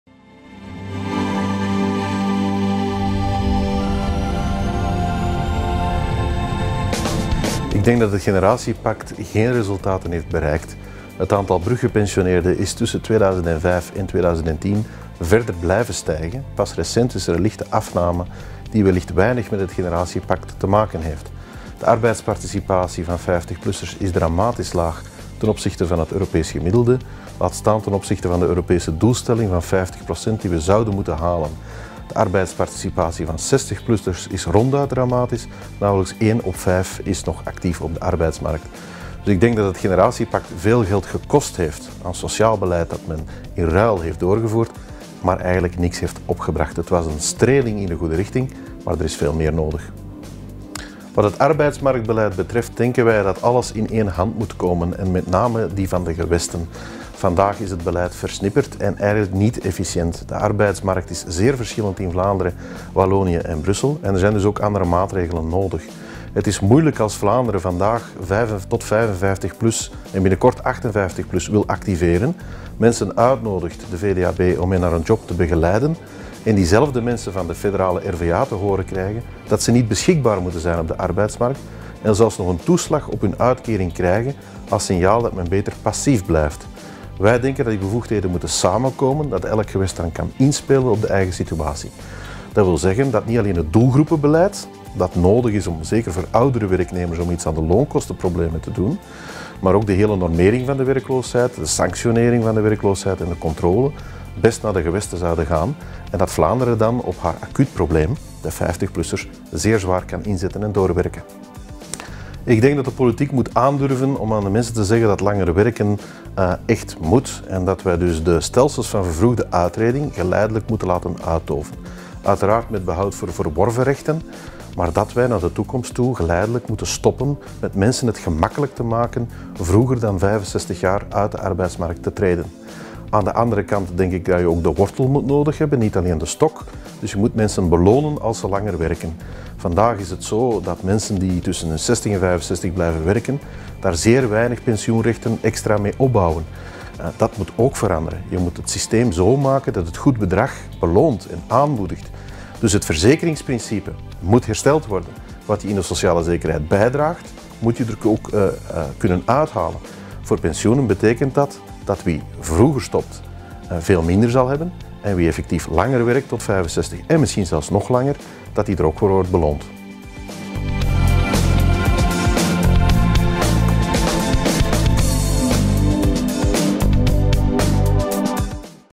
VBO FORUM 2011: 50+, experience is our capital! Videoboodschap Bart De Wever, voorzitter NVA
Voor zijn editie 2011 nodigde het VBO Forum uit om een namiddag lang mee na te denken over de onderbenutte rijkdom die 50-plussers in België vertegenwoordigen.
De tewerkstellingsgraad van 50-plussers in ons land ligt immers aan de lage kant. Door hun ervaring vormen senioren nochtans een kostbaar menselijk en professioneel kapitaal... dat om allerlei redenen vaak over het hoofd wordt gezien.